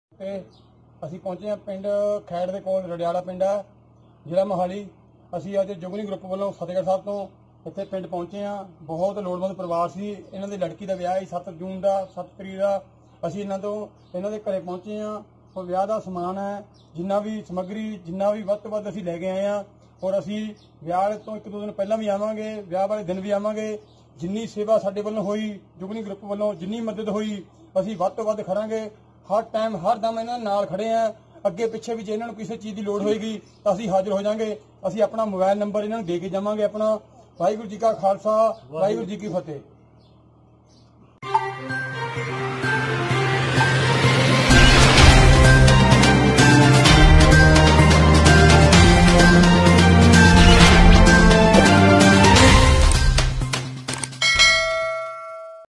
जुगनी ग्रुप कर रहा है जरूरतमंद परिवारों की सहायता !! Covered by Fast Media TV
Jugni Group is helping needy families